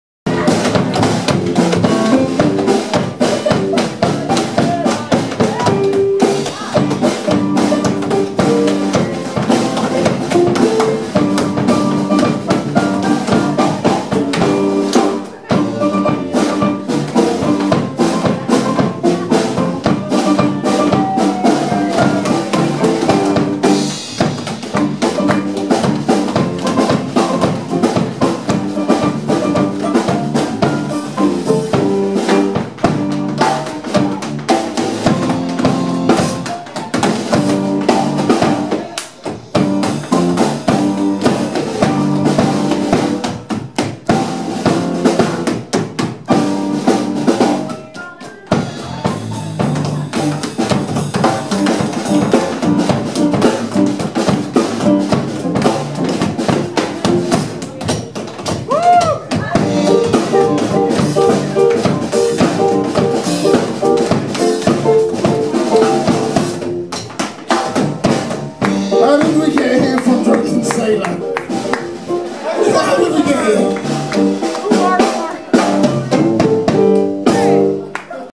Lee LTJ 26Jan2020 574
Lee Payne, London Tap Jam with the Michele Drees Trio, Gigi's Bar, Hoxton, 26 January 2020